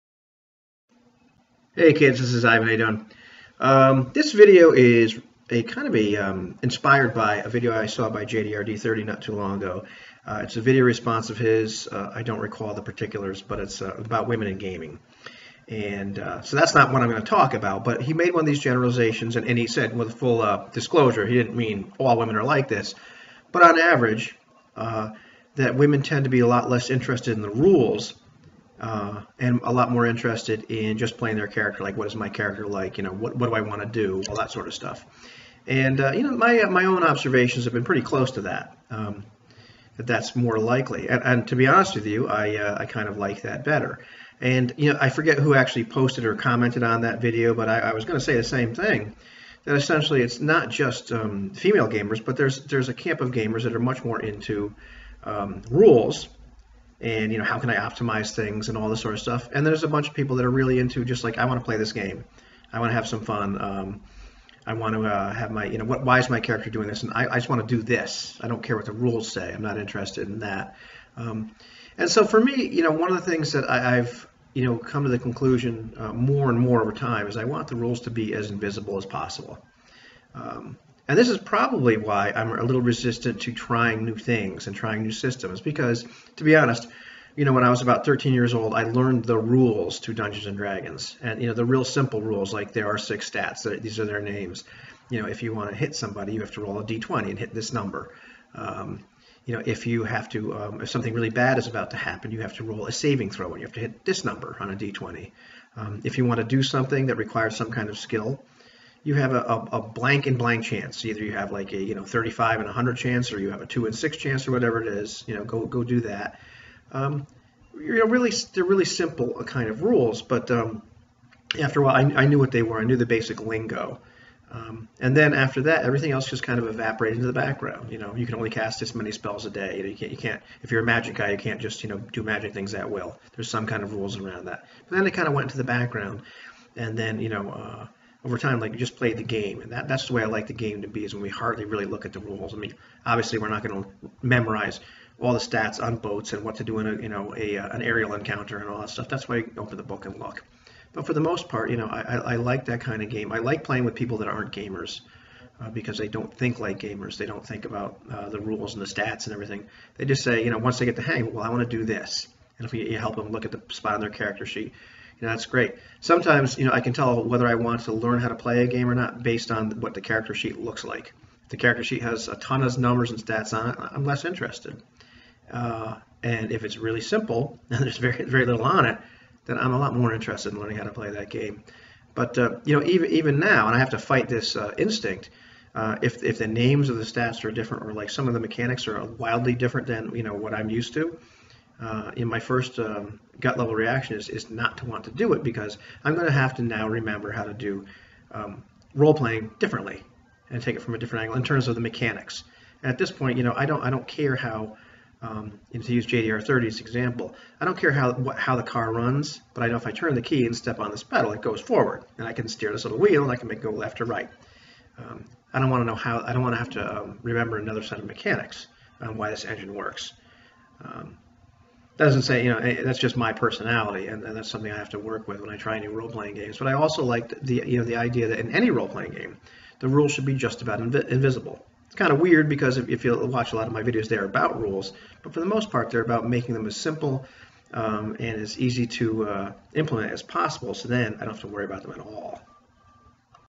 Rules should be invisible in RPGs
my rant on how up front the rules of an RPG "should" be, an admission of my resistance to change, and a look at my gaming philosophy or some such nonsense.